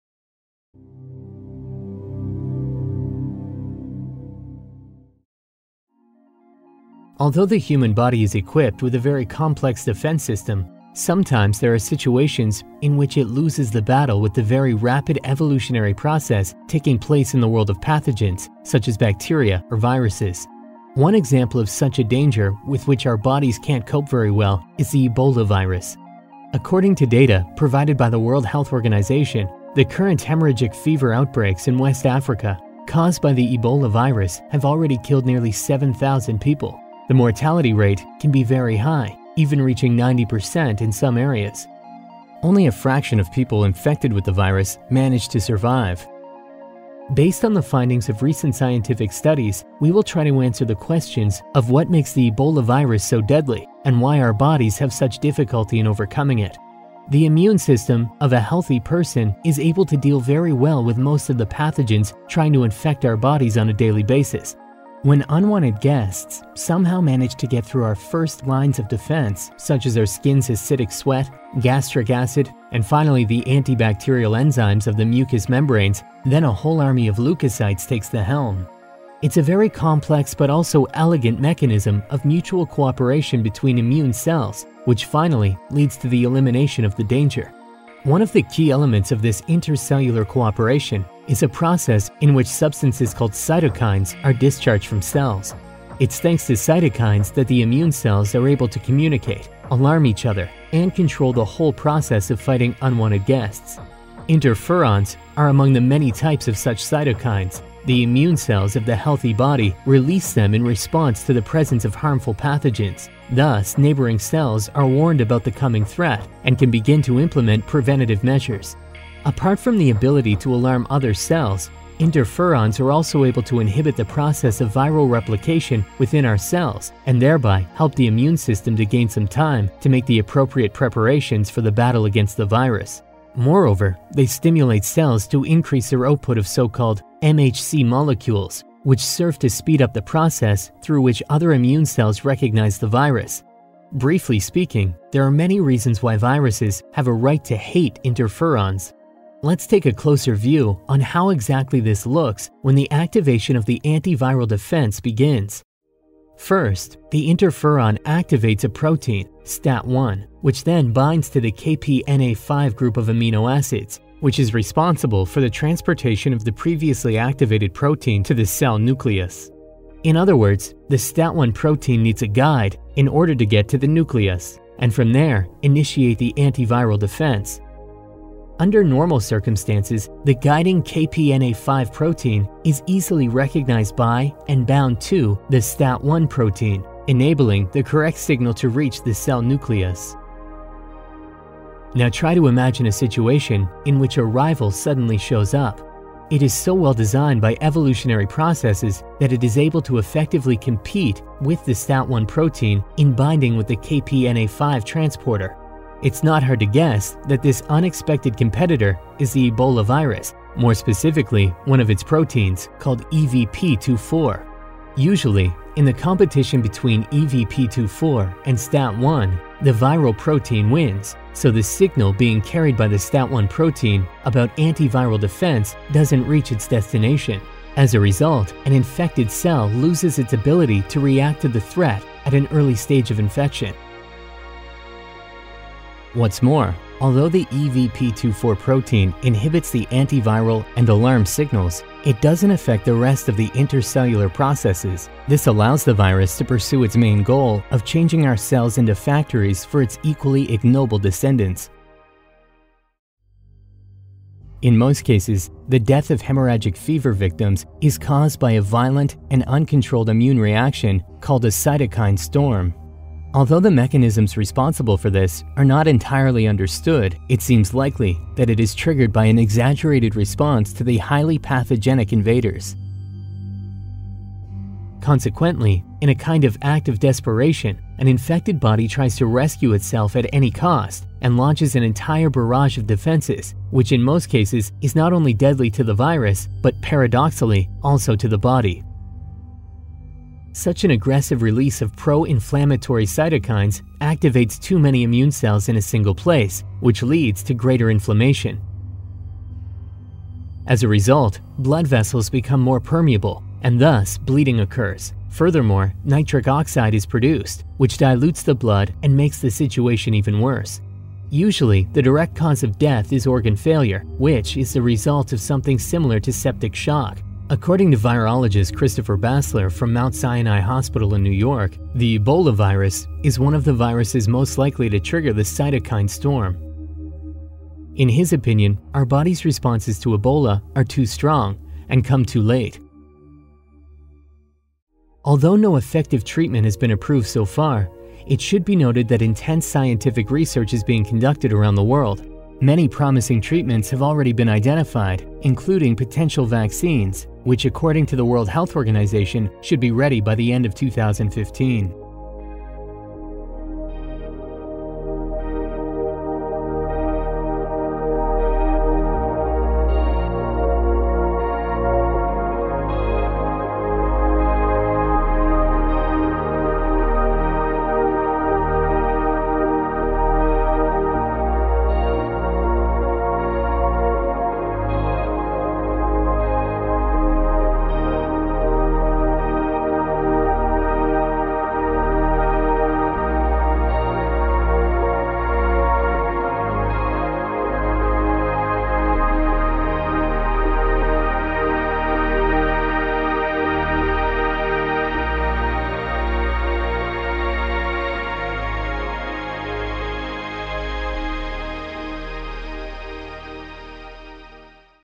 What makes Ebola virus so deadly?
What exactly makes the Ebola virus so deadly? Why our bodies have such difficulty in overcoming it? Ebola virus explained.

PLEASE TAKE NOTICE that all of the necessary information about the authors who were kind enough to share their work, are displayed in the end credits.